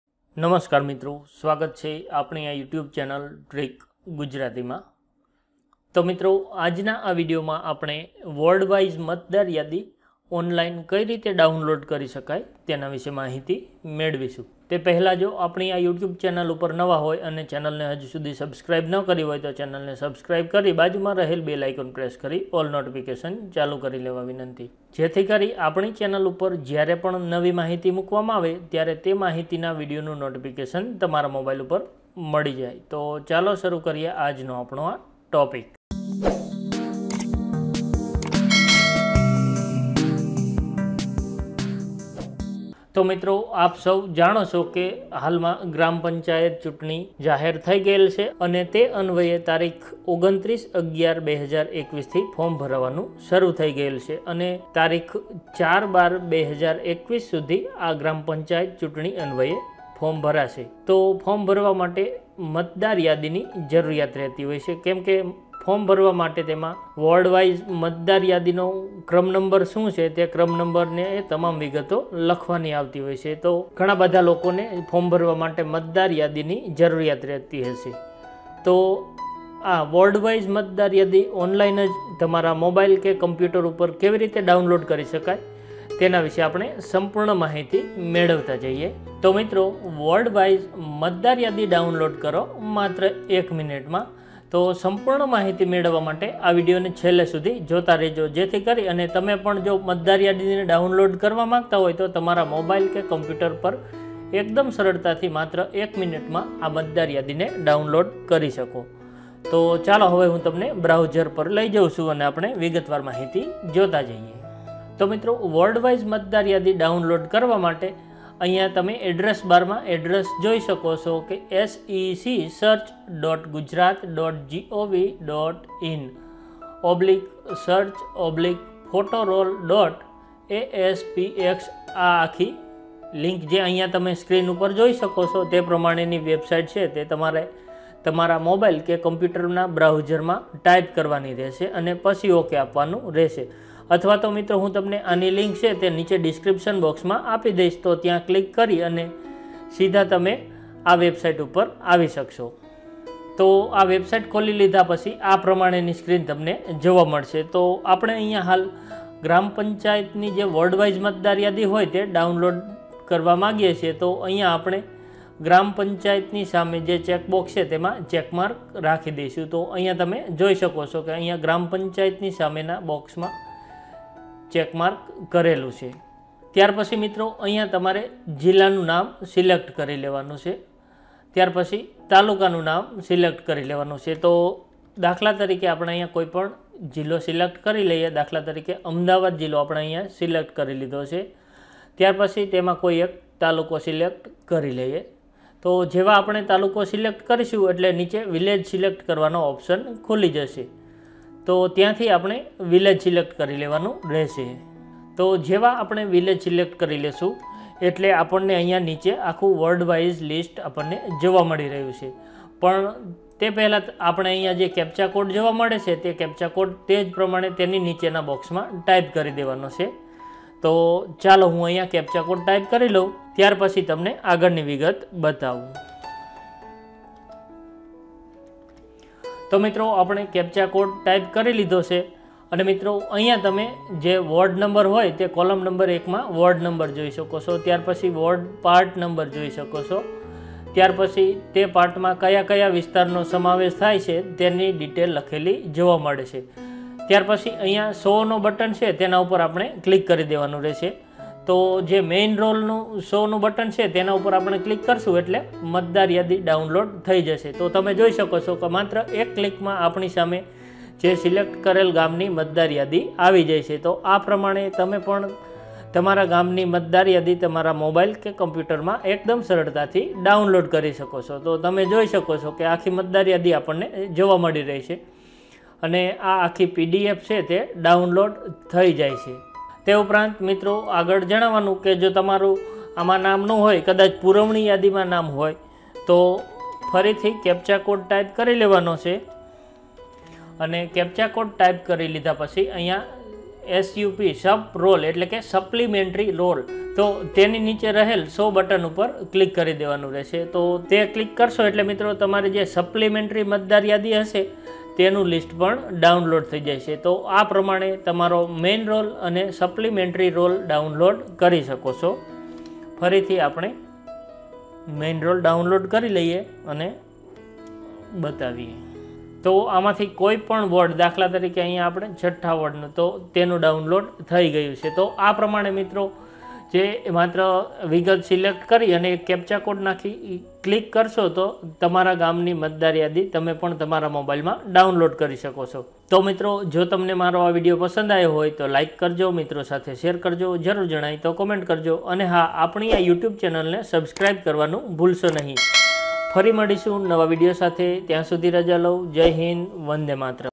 ગ્રામ પંચાયત ચૂંટણી | મતદાર યાદી કેવી રીતે ડાઉનલોડ કરવી? | How to Download Word wise Matdar Yadi
મિત્રો આ વિડયોમાં ગ્રામ પંચાયત ચૂંટણી માટે વોર્ડ વાઇઝ મતદાર યાદી કેવી રીતે ડાઉનલોડ કરવી તેના વિશે માહિતી આપવામાં આવેલ છે

મતદારયાદી ડાઉનલોડ કરવા માટેની લીંક :-